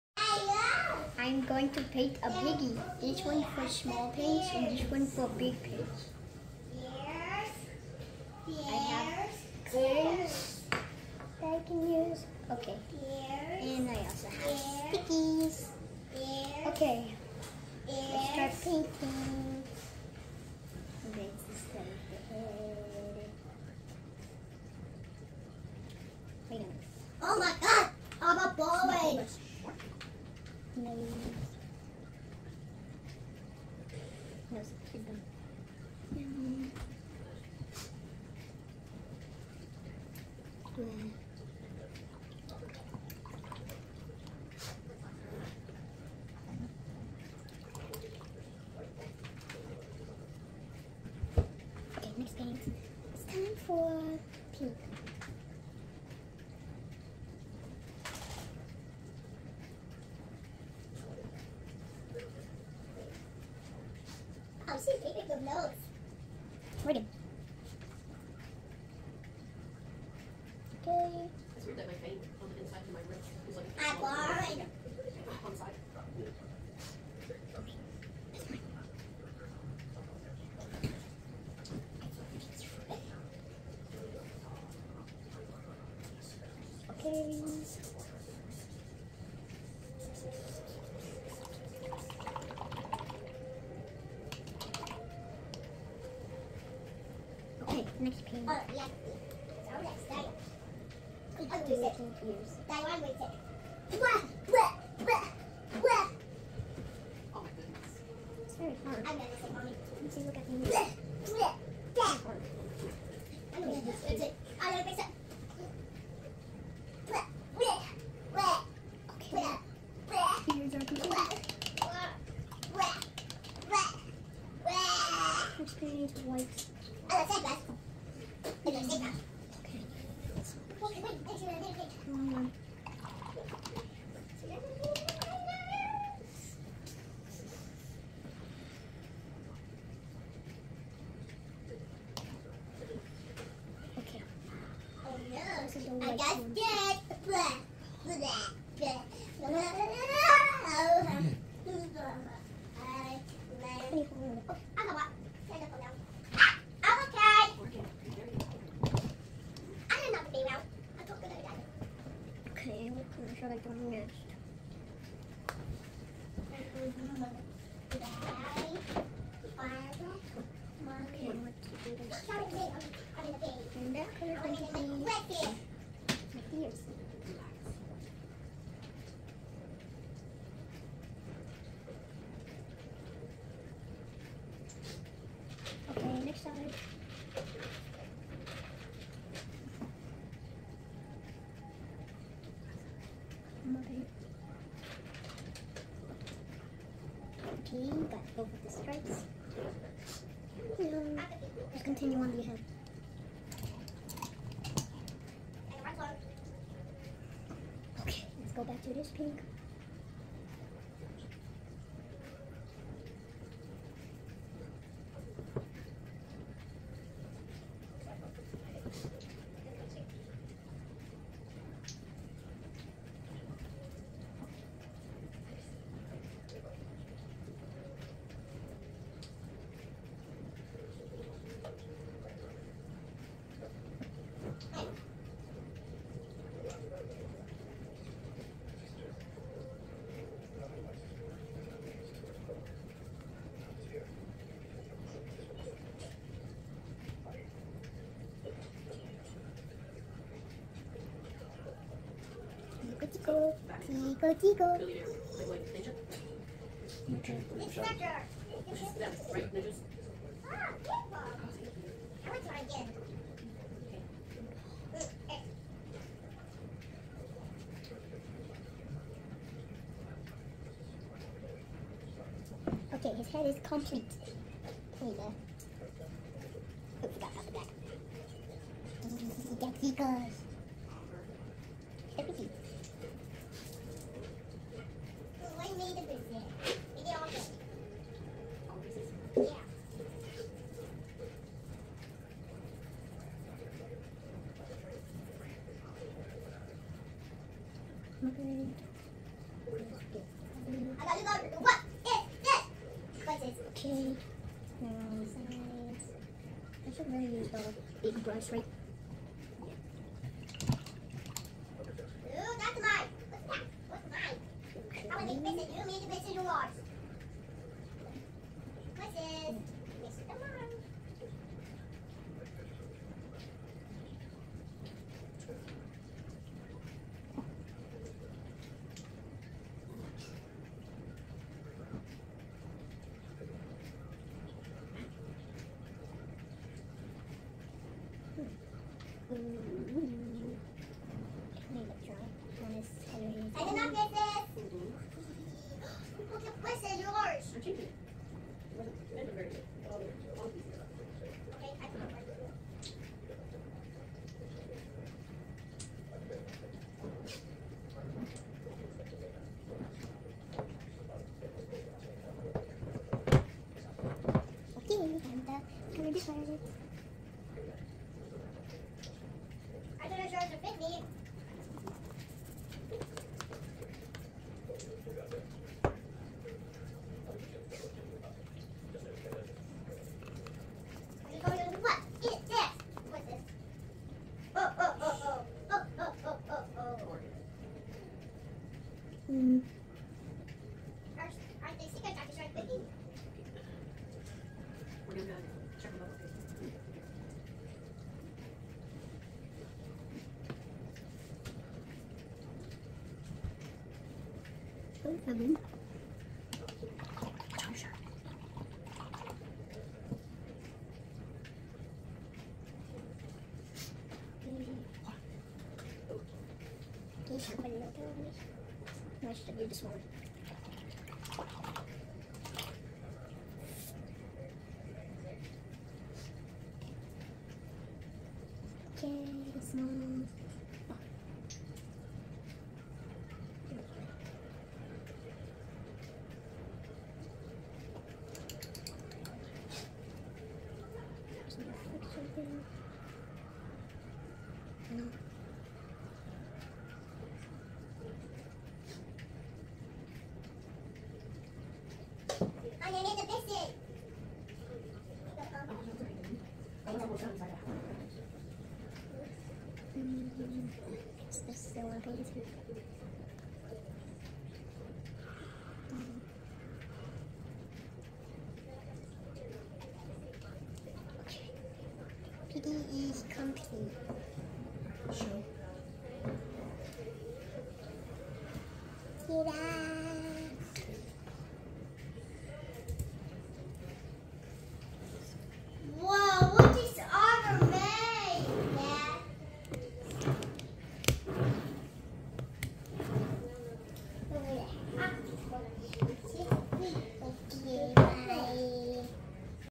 Paint the Minecraft pig (piggy bank) with me!
Watch me paint the minecraft pig as a piggy bank.